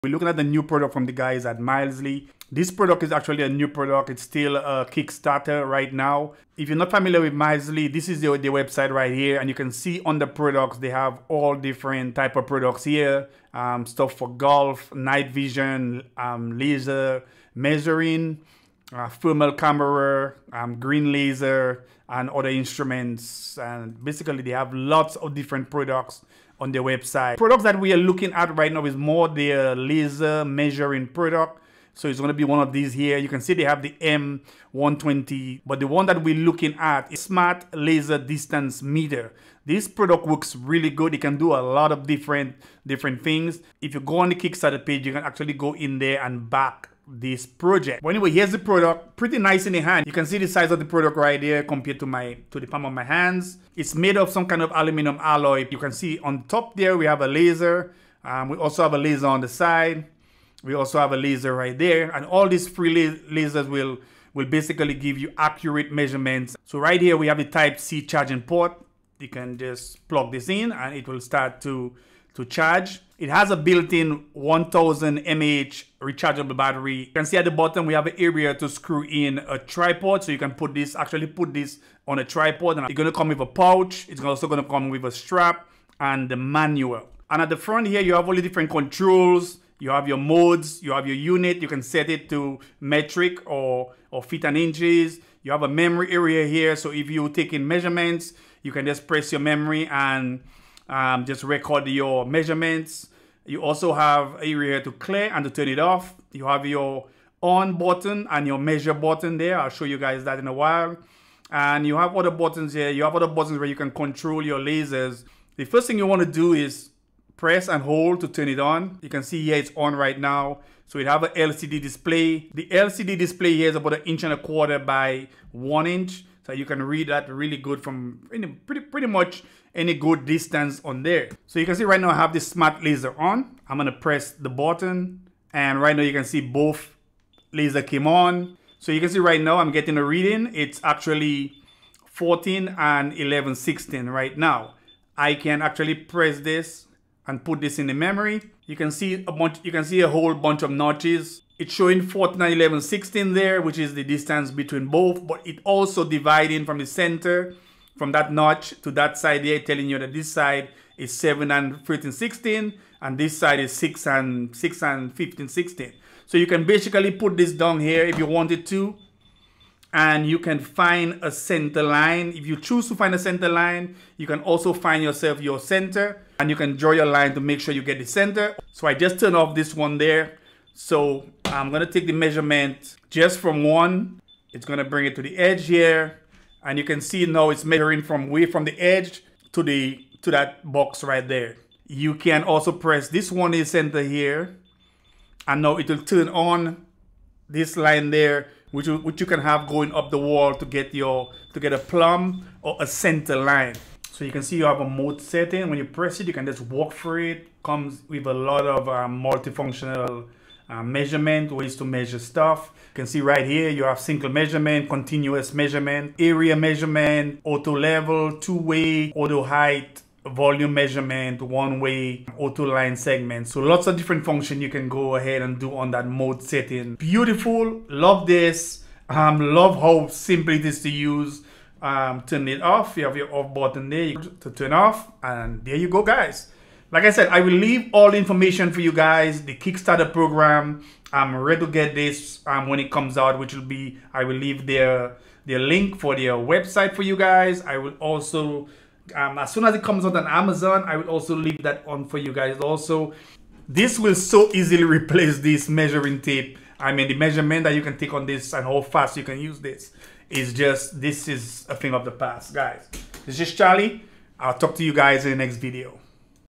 Smart Bilateral Laser Measuring Meter DP20 PRO Mileseey
DP20 PRO is an exquisite bilateral laser distance meter whose exterior redefines the concept of industrial design—the cuboid-shaped body with thin surface and low-profile key caps renders a product appearance as simple as a ruler. It involves upgraded and patented distance-measuring technology that enables more efficient and accurate measurement, which meets various measurement needs of people engaged in house renovation, house decoration, interior design, exhibition design, outdoor advertising design, crafting, home soft furnishing, etc